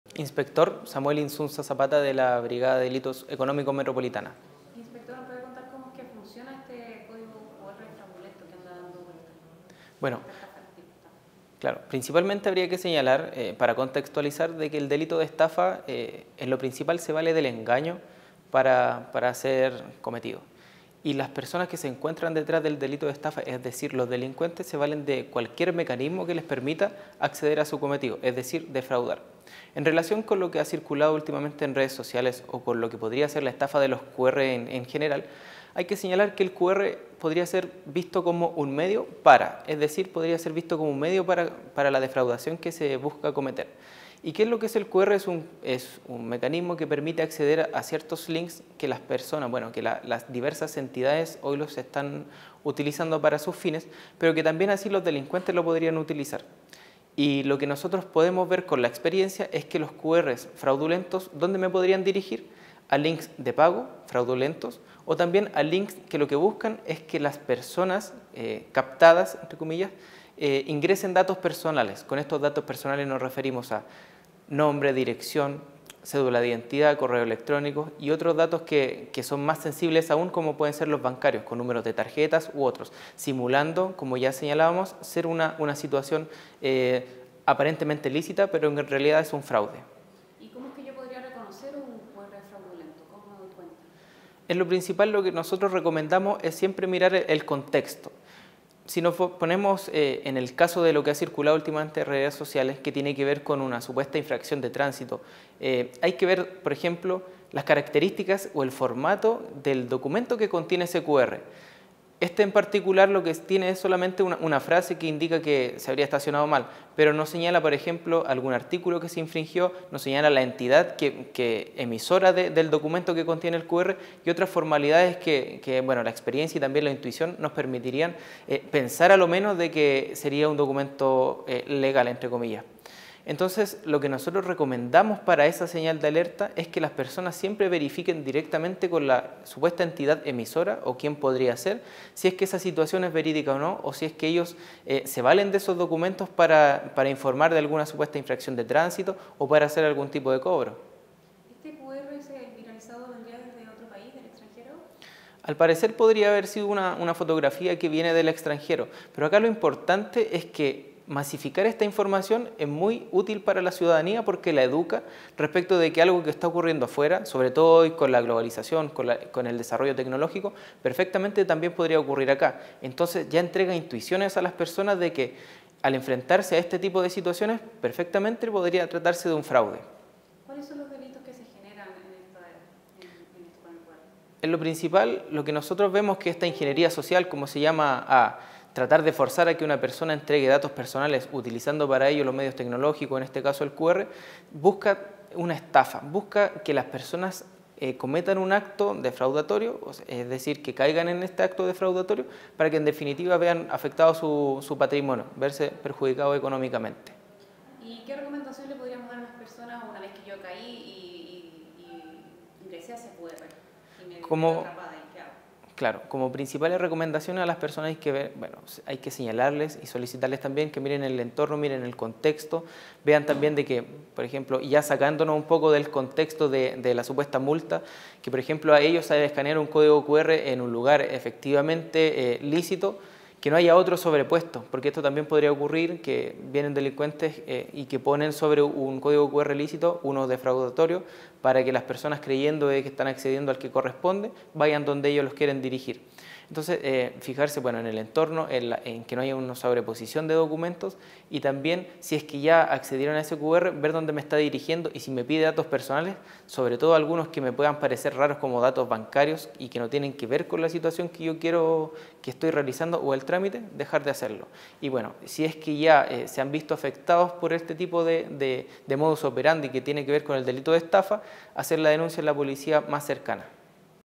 Inspector de la PDI advierte sobre un supuesto Código QR en redes sociales 🚨📱
El Inspector Samuel Inzunza, de la Brigada Investigadora de Delitos Económicos Metropolitana, se refirió a la circulación de un presunto Código QR relacionado con infracciones de tránsito.

🔎 Se recomienda a la ciudadanía estar alerta y verificar la autenticidad de este tipo de información para evitar posibles fraudes.